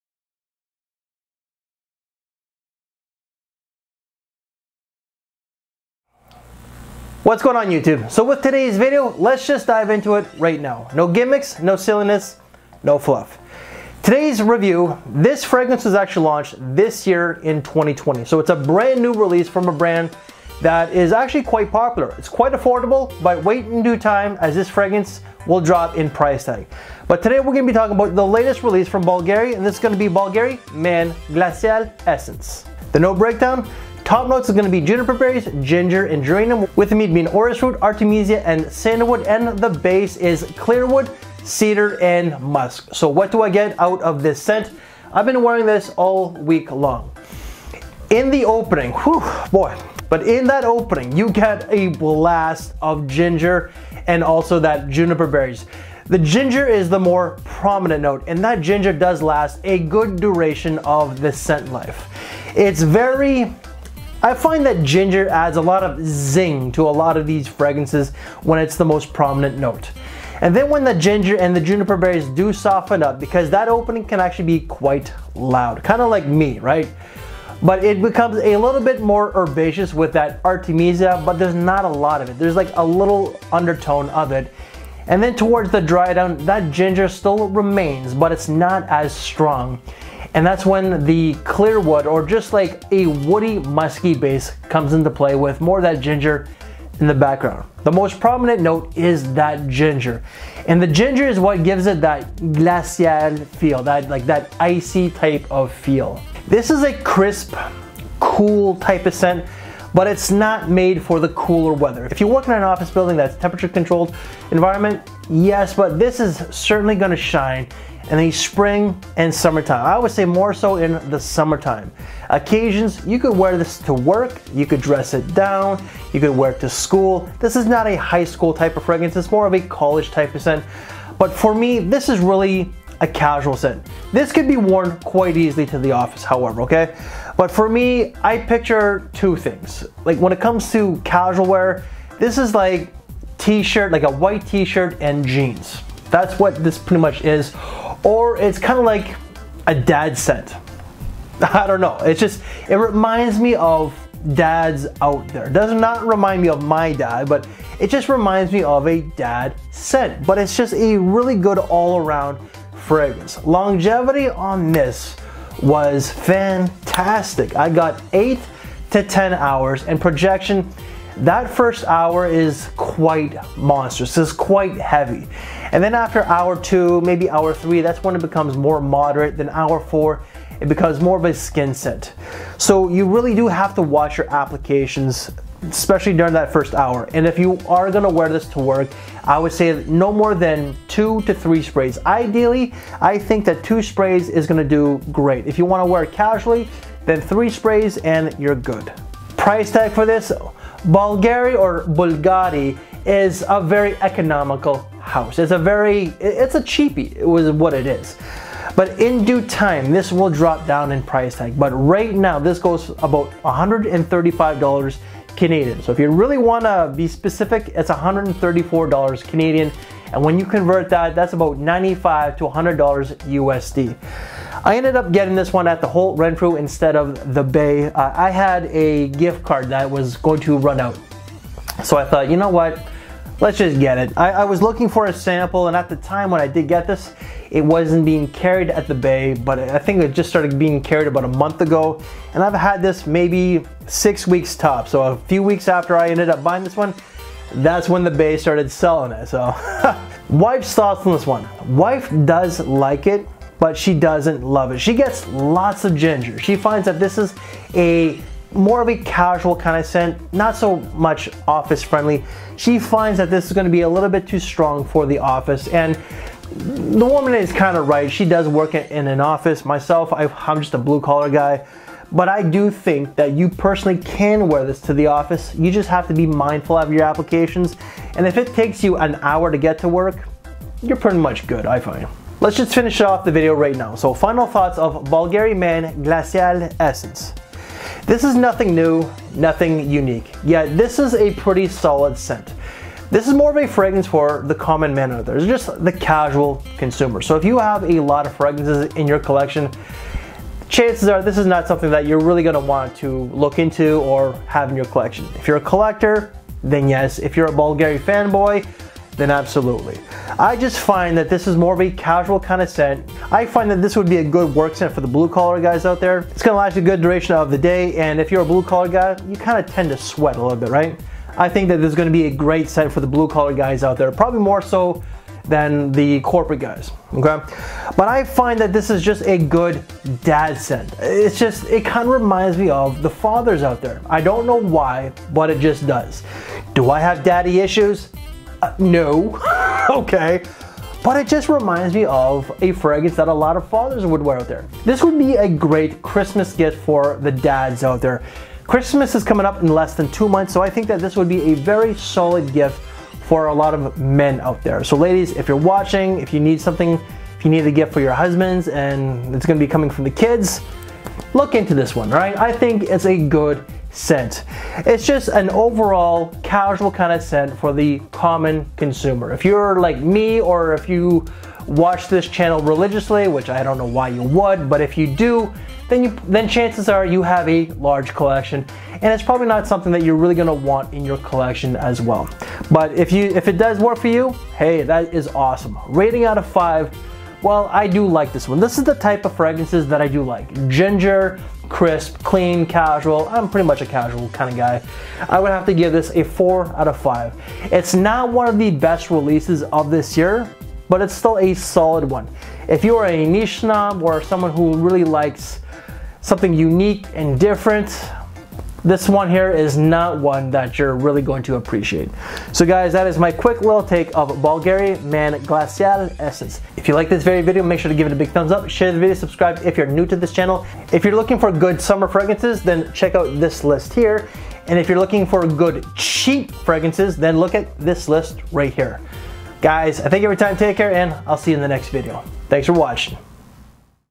SUMMER GEM - Bvlgari Man Glacial Essence fragrance/cologne review
Bvlgari Man Glacial Essence was launched in 2020 and this is one that I think will be a summer banger.